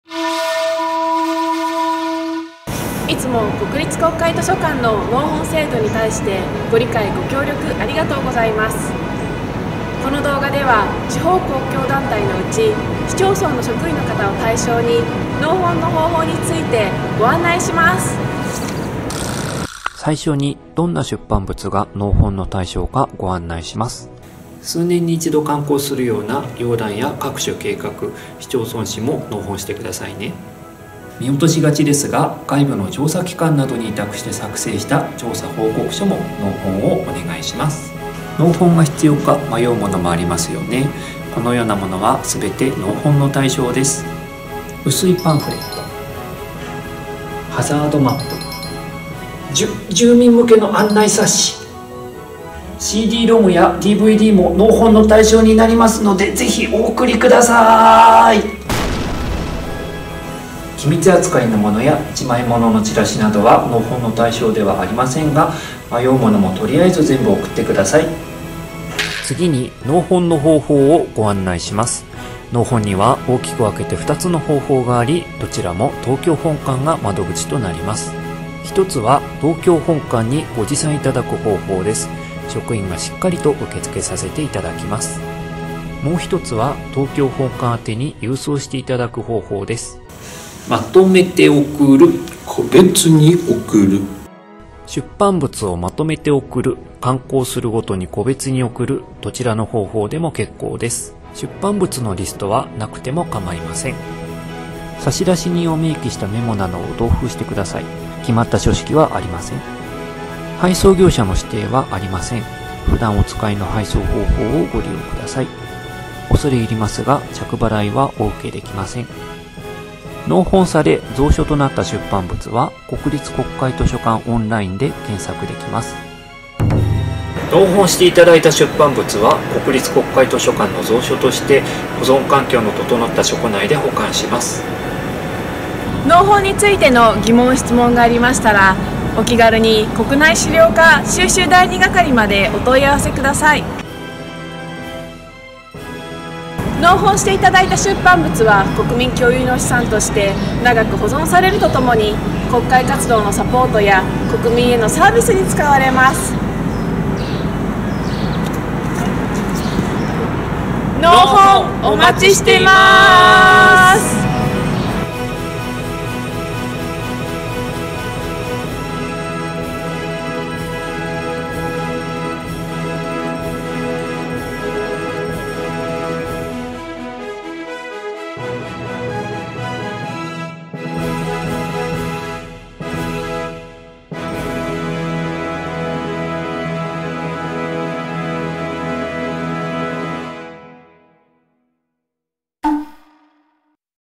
国立国会図書館 納本のご案内（地方公共団体の皆様へ）
地方公共団体の職員の方向けに、納本の方法をご案内します。
国立国会図書館東京本館の館内紹介もしています。
詳細は下記をご参照ください。

国立国会図書館
収集書誌部 国内資料課 収集第二係

●納本資料の送り先、納本対象となる資料についてはこちらから

●納本制度の概要についてはこちらから

※お送りいただいた資料の取り扱いについては、当館にご一任ください。

※資料掲載にご協力いただいた機関
西興部村／川崎市／林野庁／高知県立のいち動物公園／さいたま市／大田区／埼玉県／横浜市／港区教育委員会／総務省情報通信政策研究所／東京都／復興庁／相模原市教育委員会／揖斐川町揖斐川歴史民俗資料館／富山県総合デザインセンター／札幌市／新国立劇場運営財団／アーツ前橋／札幌市北保健センター／札幌市／国立文化財機構 文化財防災センター／明和町／宮城県総合教育センター／小郡市教育委員会／長与町／八郎潟町教育委員会／狛江市 （掲載順）
ご協力、ありがとうございました。